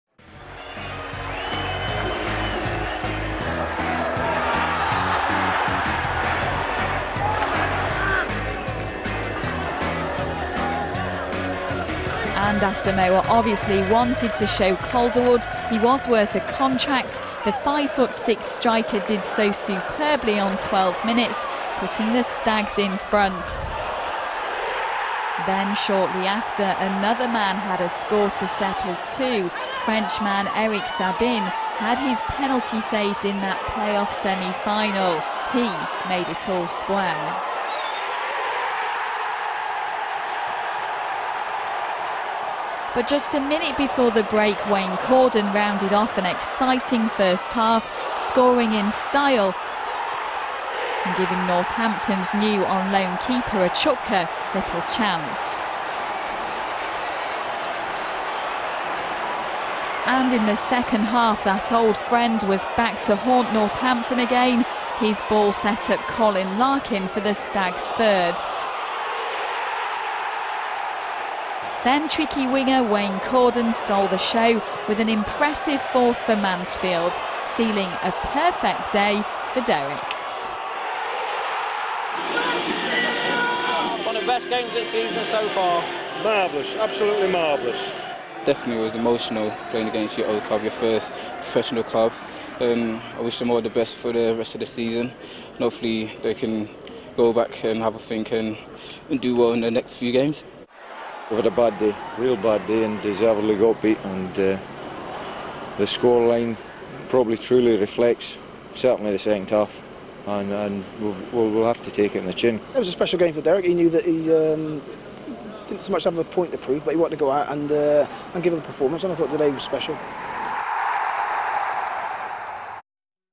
The Stags beat Northampton 4-1 in September 2004
Mansfield Town 4-1 Northampton Town, 05Sep2004